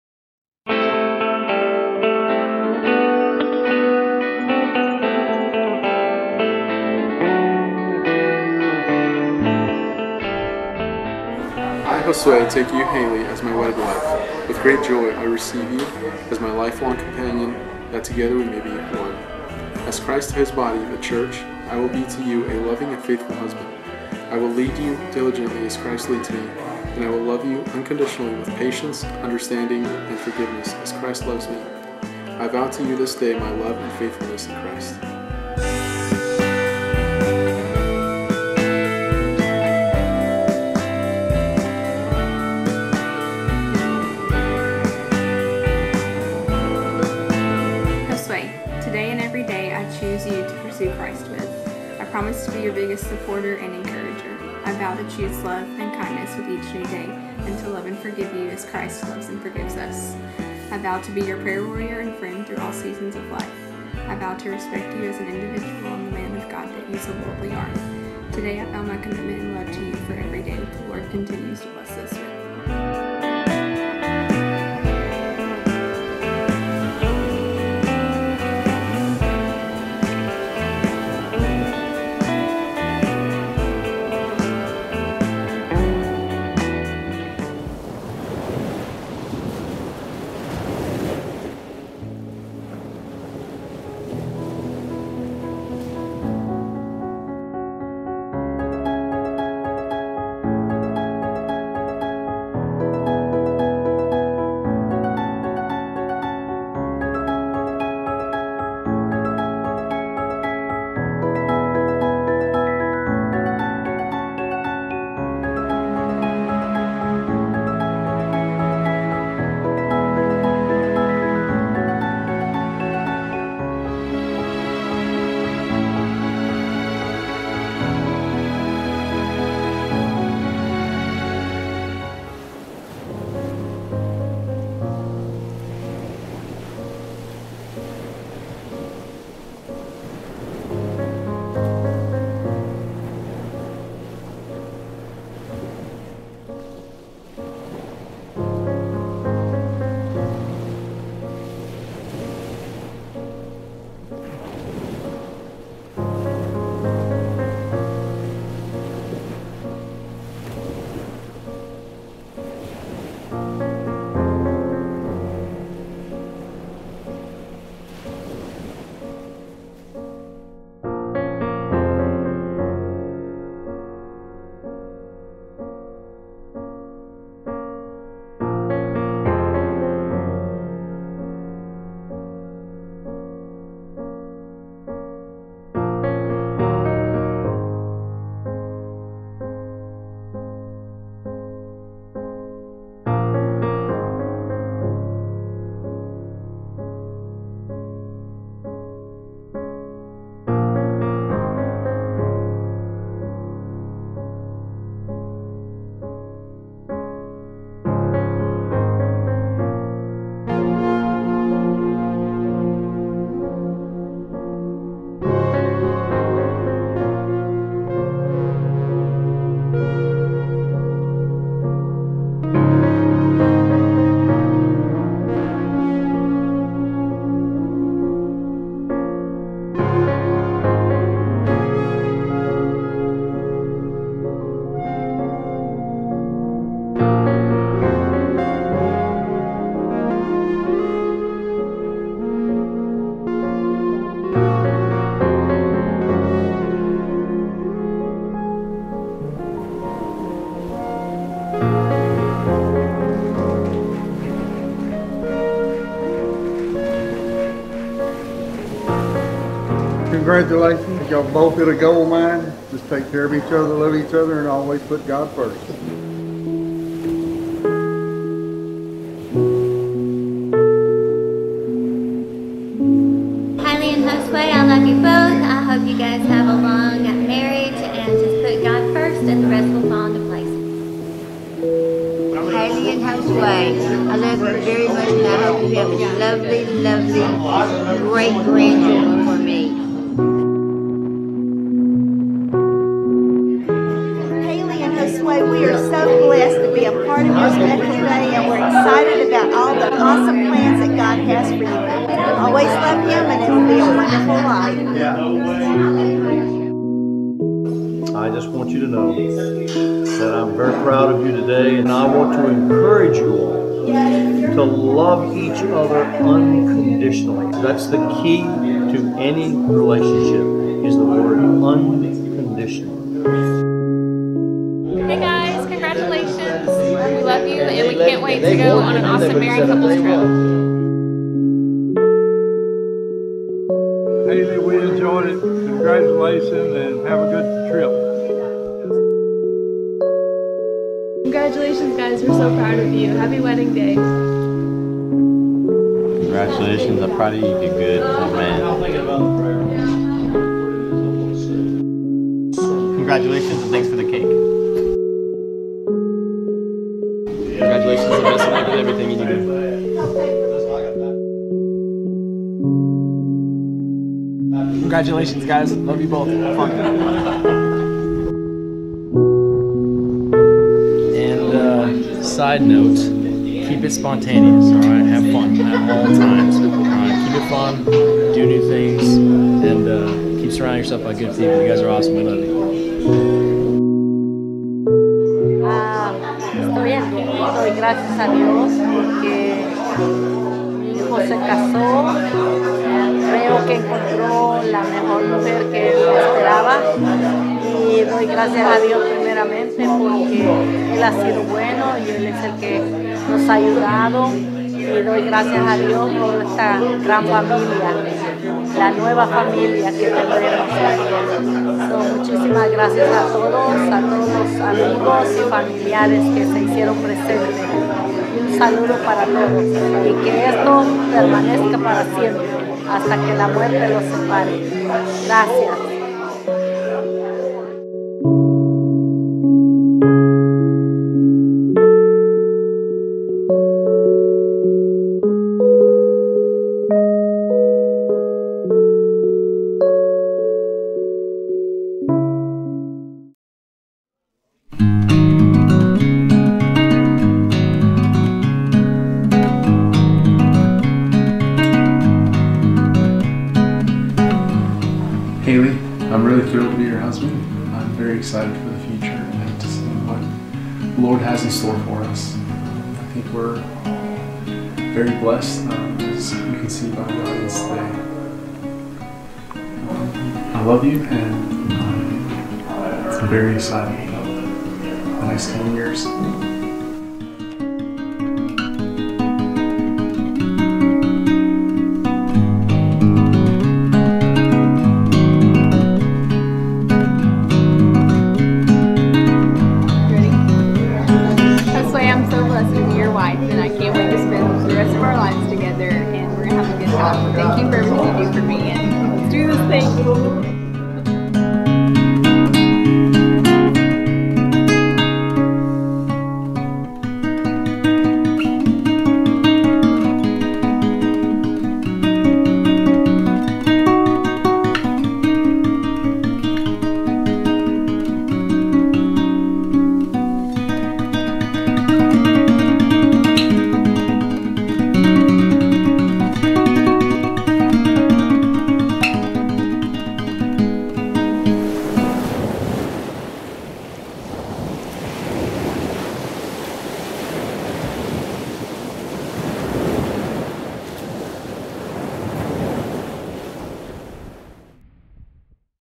Long Walk on the Beach
Here's to new beginnings and my first upload of the year!  I've missed a couple vlogs because I've been working on this for the newlyweds.   Best wishes, Josue' and Hayley, I hope you guys enjoy it!

Music (in order) by:

Jahzzar
Dexter Britain
Chris Zabriskie
Podington Bear
Gillicuddy